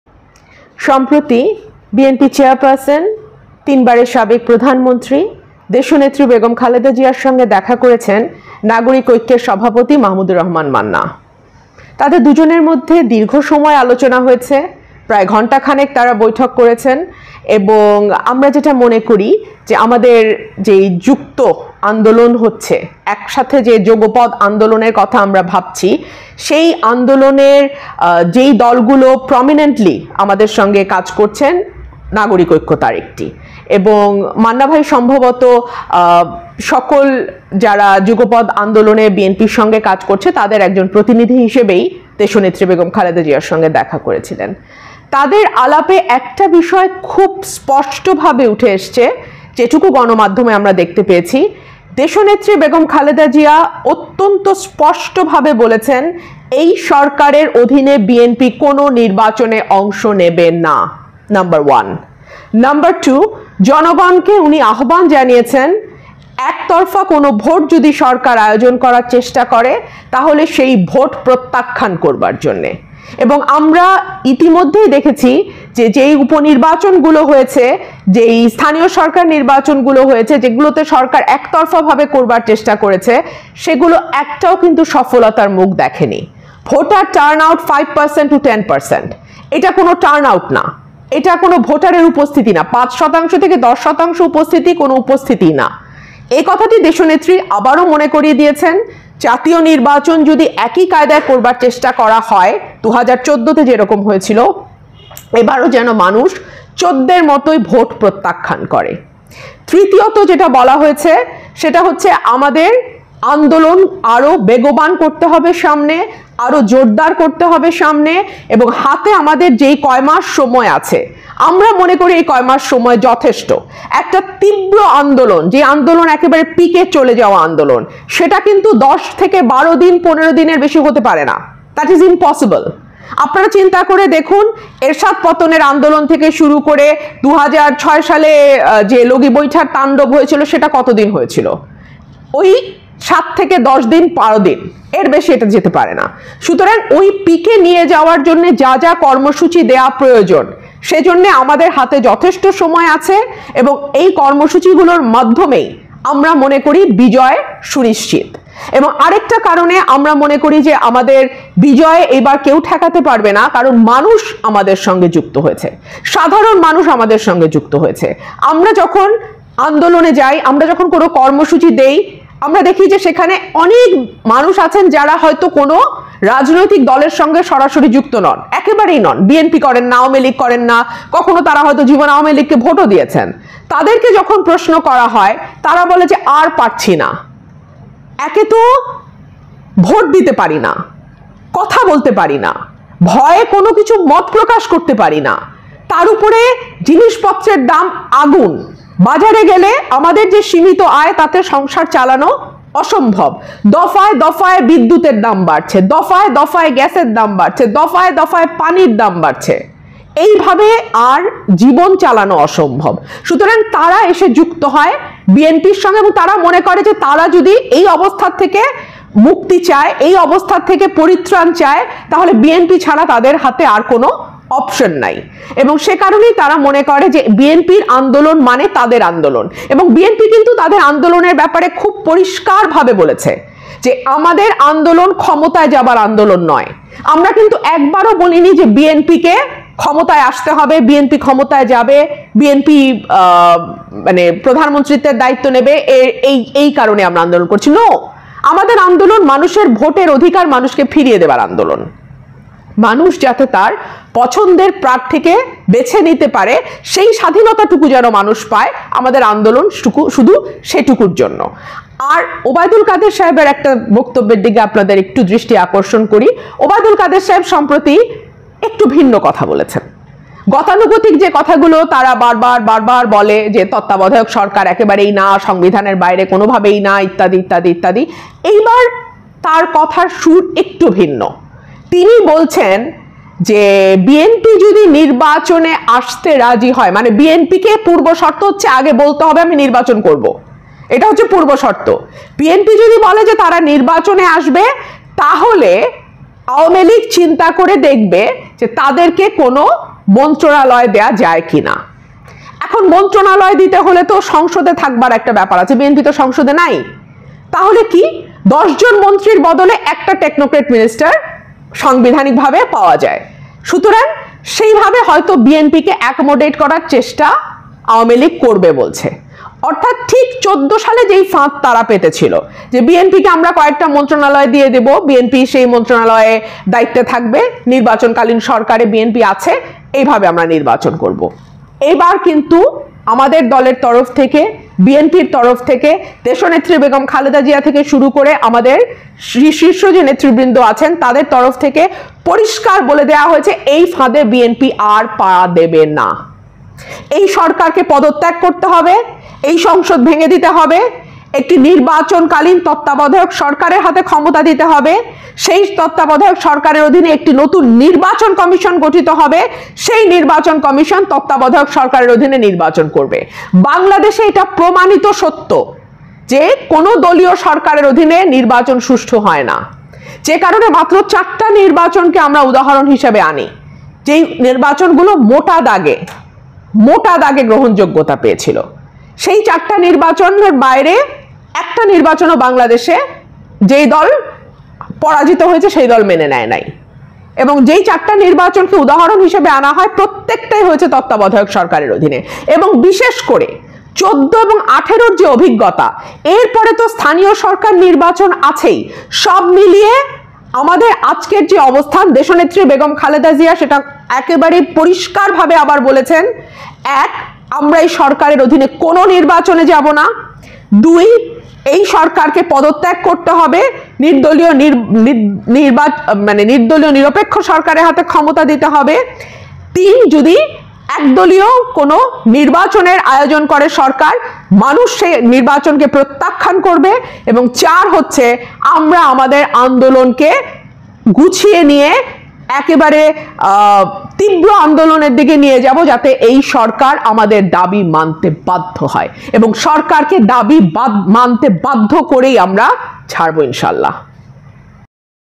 এই সরকারের অধীনে নির্বাচন নিয়ে আর কোনো আলাপ অবান্তর । Rumeen's Voice । রুমিন ফারহানা
নাগরিক ঐক্যের সভাপতি মাহমুদুর রহমান মান্নার সাথে সাক্ষাৎ করে দেশনেত্রী বেগম খালেদা জিয়া এই সরকারের অধীনে যে কোন ধরনের নির্বাচনের সম্ভাবনা নাকচ করেছেন। এরপর নিশ্চয়ই এটা নিয়ে আর কোন রকম কোন কথা বা আলোচনা থাকতে পারে না।